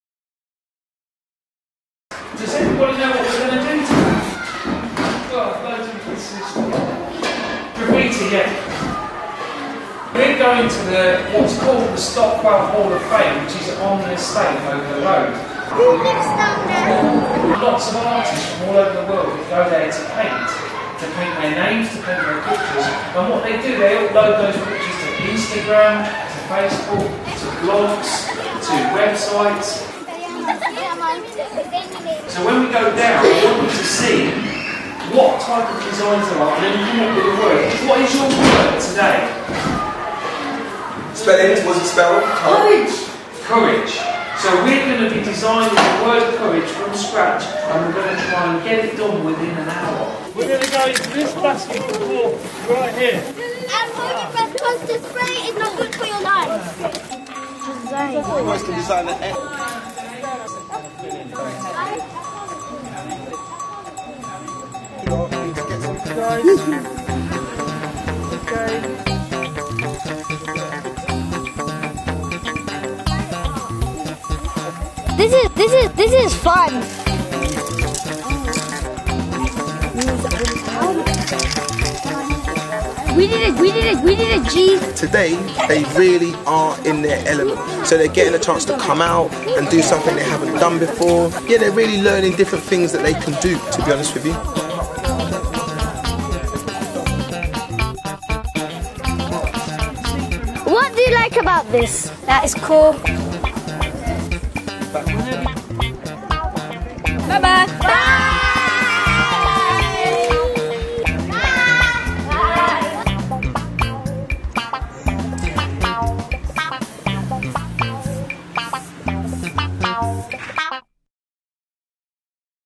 PAINTED BRIXTON SCHOOL 3&4
Painted Brixton School session 3 and 4 - twice the work in half the time - year 4 complete their graffiti experience at Stockwell Hall of Fame. We hear from their teachers, and see their work.
Thanks to Stockwell Community Centre.
Funded by Big Local North Brixton and London Community Foundation.